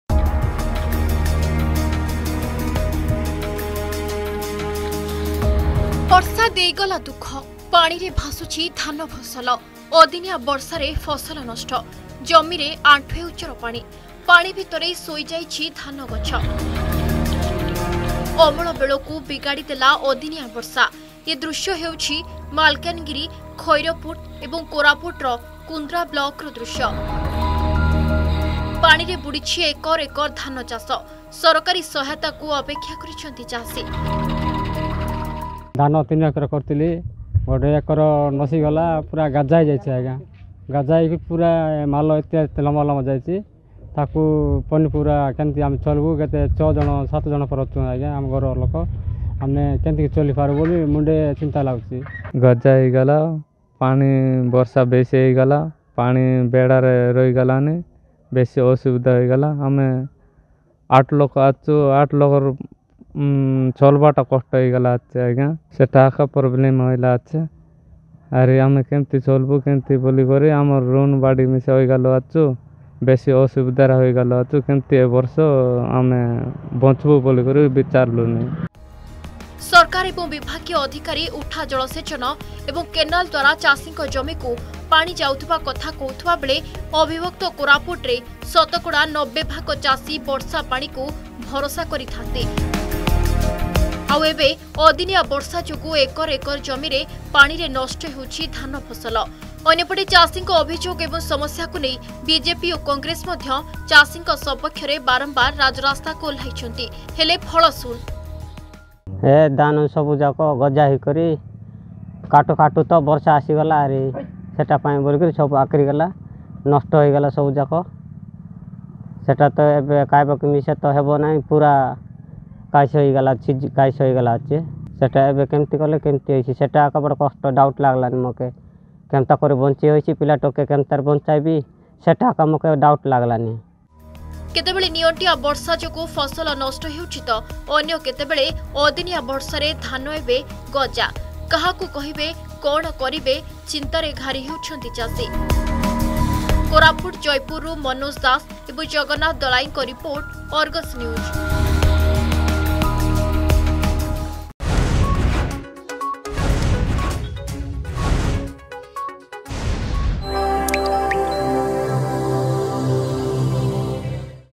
ବର୍ଷା ଦେଇଗଲା ଦୁଃଖ
ପାଣିରେ ଭାସୁଛି ଧାନ ଫସଲ
ଅଦିନିଆ ବର୍ଷାରେ ଫସଲ ନଷ୍ଟ
ଲଗାତାର ରାଜ୍ୟ ସରକାର ଚାଷୀଙ୍କ ସୁବିଧା ପାଇଁ ମାଳମାଳ ଯୋଜନା ଆଣୁଛନ୍ତି କିନ୍ତୁ ସରକାରୀ ସହାୟତାରୁ ବଞ୍ଚିତ ଚାଷୀ । ଅଦିନିଆ ବର୍ଷାରେ କୋରାପୁଟରେ ଏକର ଏକର ଜମିରେ ନଷ୍ଟ ହେଉଛି ଧାନ ଫସଲ ।  ଯାହାକୁ ଦେଖି ମୁଣ୍ଡରେ ହାତ ଦେଇଛି ଚାଷୀ ।